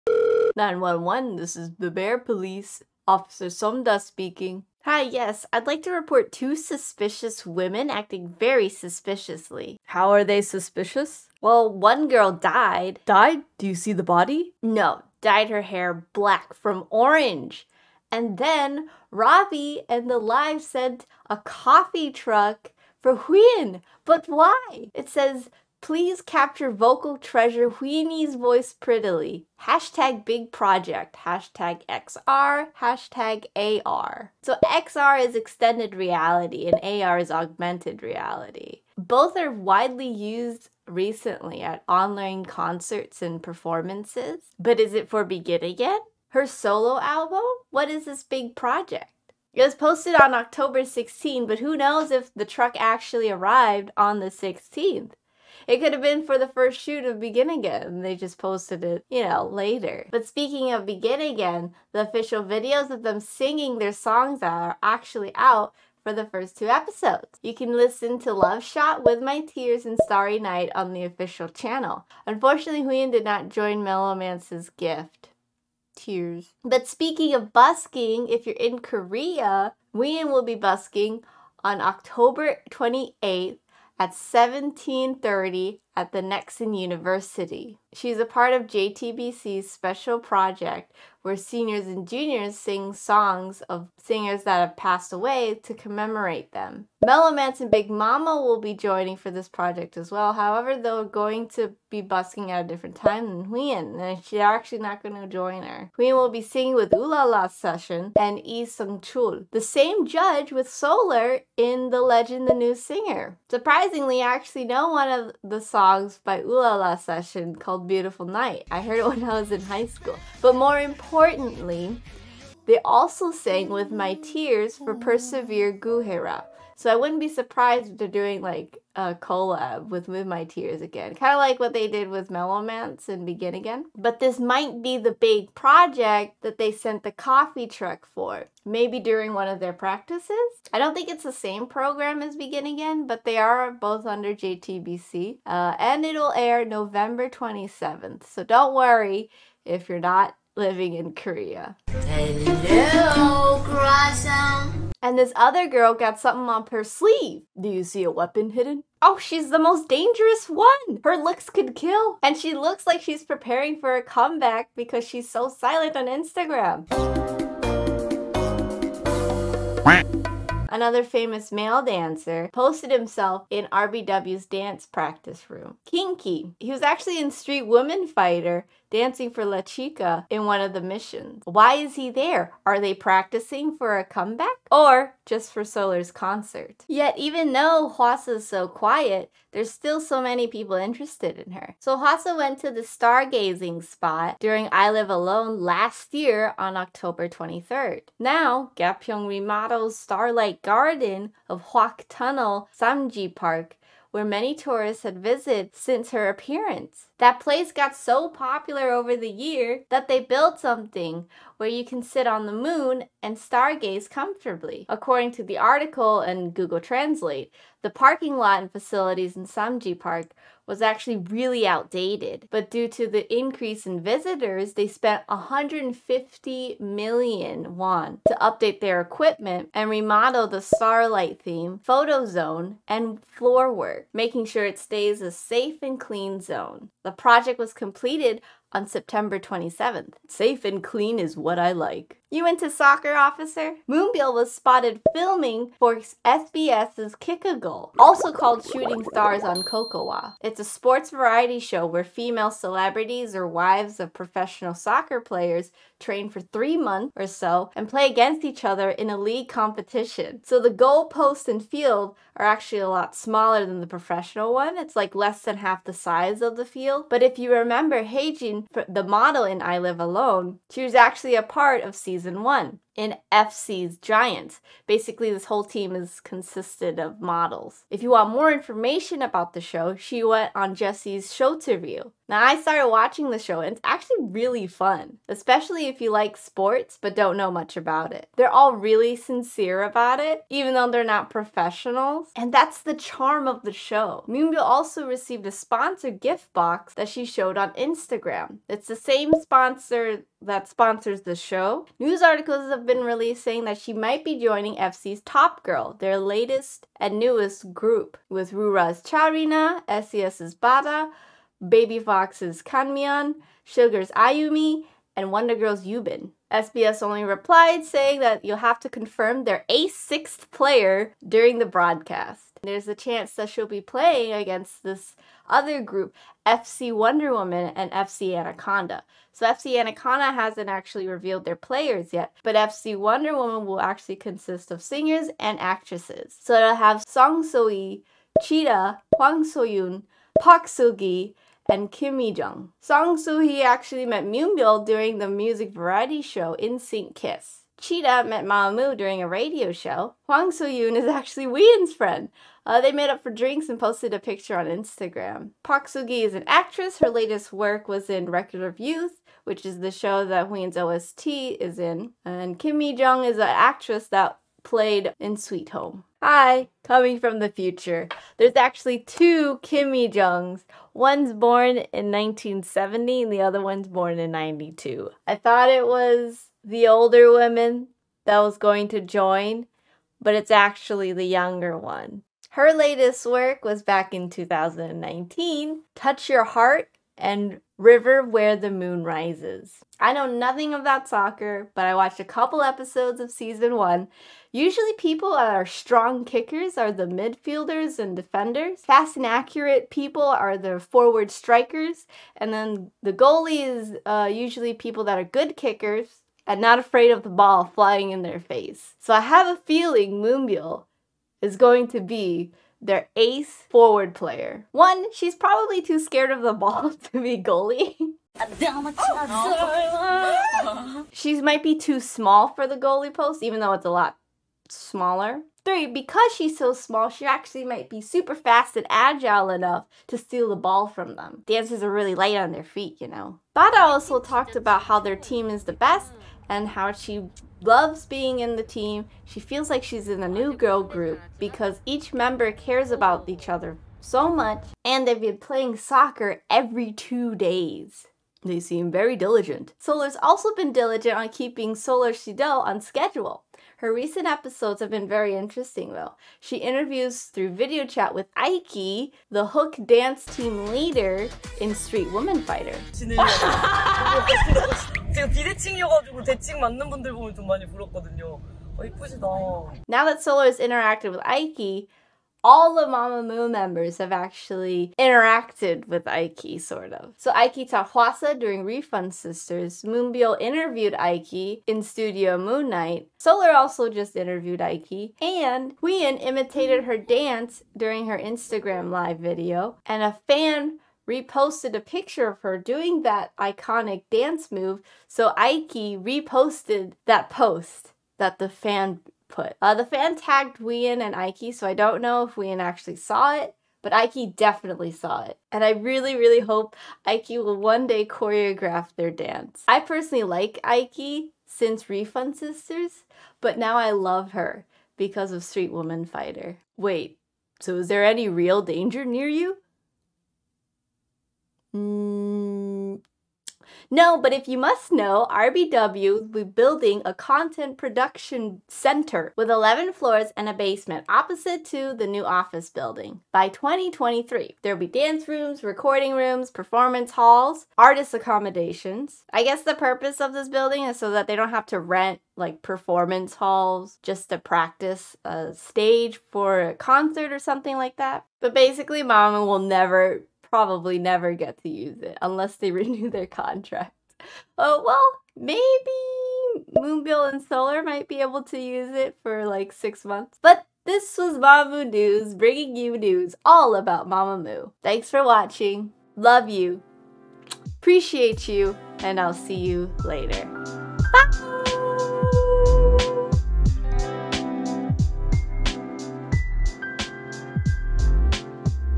Moonbyul Training for Soccer, Aiki x Mamamoo, Wheein Busking on 10/28/21
Wheein dyed her hair, Wheein got a coffee truck from Ravi, Wheein will be busking on 10/28/21 in Korea, Kinky is in RBW's dance practice room, Solar interviewed Aiki from Hook dance team, Hwasa revitalized a park just by visiting it to star gaze last year, Moonbyul will be joining a new variety show for a whole season, RBW will be building a new building by 2023.

Chapters:
00:00 Intro
00:15 Wheein Busking
2:35 Hwasa and her Effect on Korea
4:01 Moonbyul as a Soccer Player
7:29 Aiki and Mamamoo
8:37 RBW's New Content Production Center

Thursday  6pm CST
Friday        6pm CST
Saturday   6pm CST

Follow @MamamooCharts for fast updates on Mamamoo comeback
Follow me @NynahNina

Sources:
Eng Sub Videos: